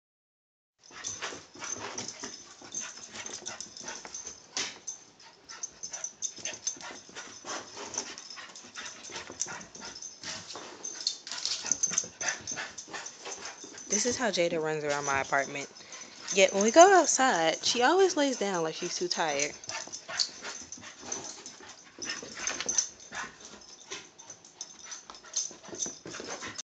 Jada running around
My dog acting a fool in my apartment